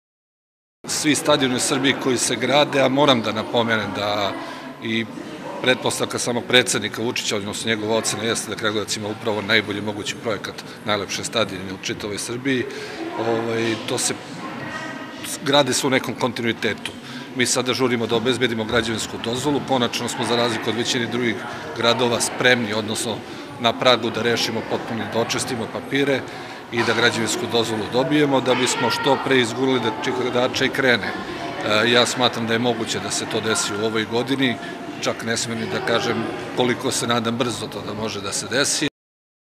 Dašić: Grad će obezbediti sve uslove za rekonstrukciju „Čika Dače“
Gradonačelnik Kragujevca, Nikola Dašić, je u izjavi za Radio televiziju Kragujevac potvrdio da će odluke, koje bi Skupština grada sutra trebalo da usvoji, ubrzati rekonstrukciju stadiona „Čika Dača“.